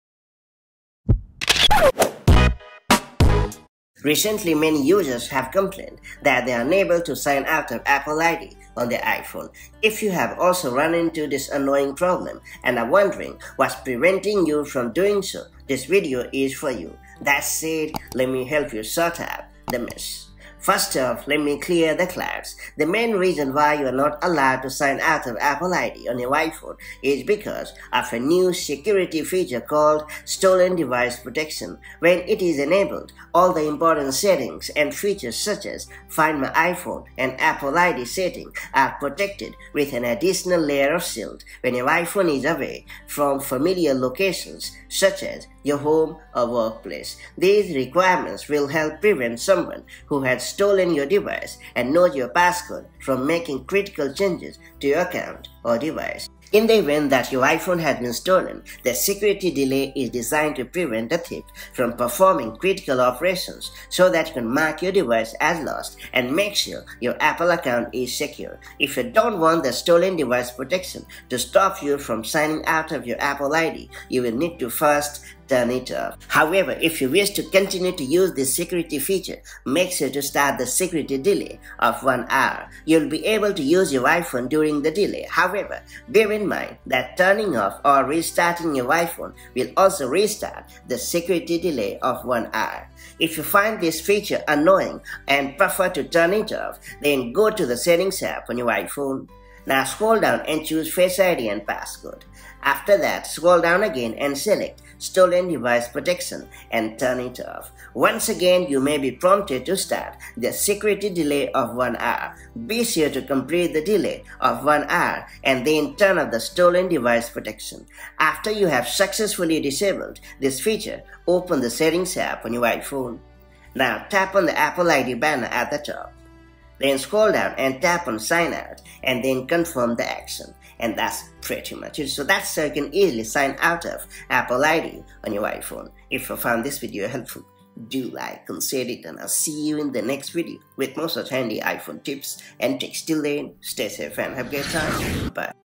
Can't Sign Out of Apple ID on Your iPhone? Here's Why!
Are you unable to sign out of Apple ID on your iPhone? Watch this video to find out the exact reason why you are not allowed to sign out of Apple ID on your iPhone and fix the problem.